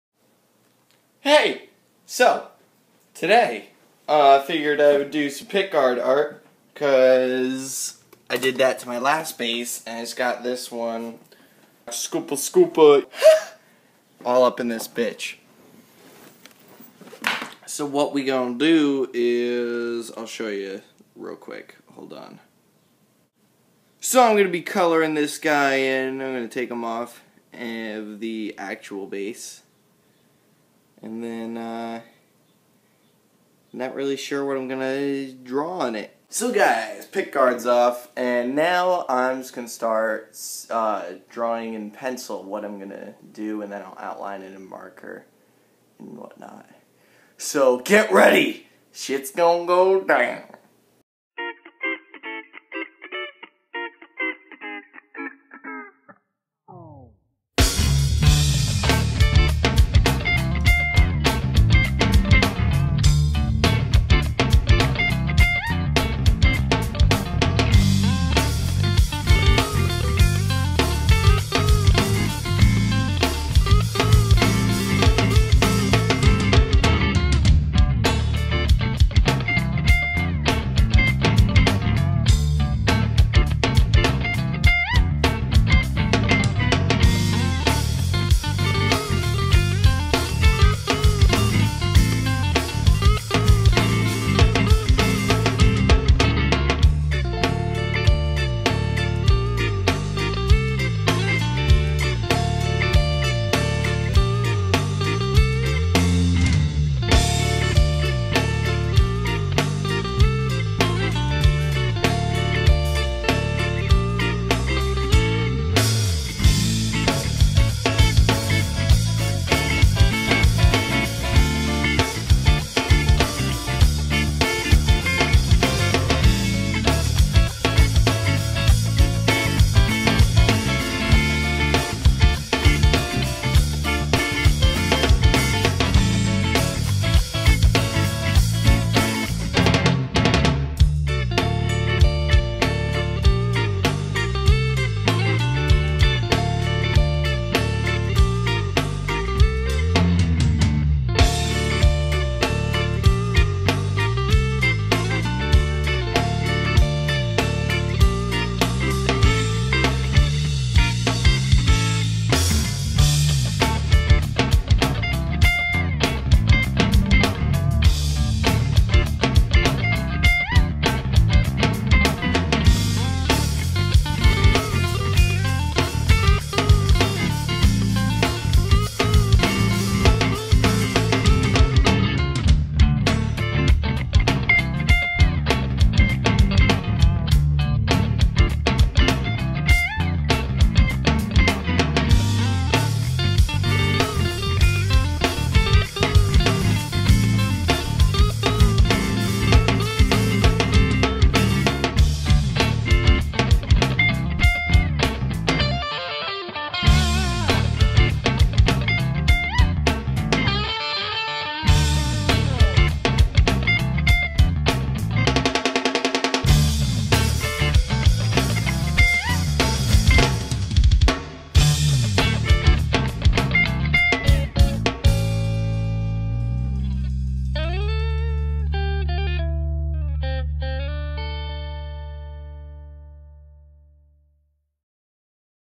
Lefty Sharpie Art: Pickguard
So I felt like drawing on my bass today because it looked a little boring. Enjoy this sped up process of my sharpie art. it's very cartoony, but I'm very happy with it :)

The song is by The Lights Beneath. A local band from Trenton NJ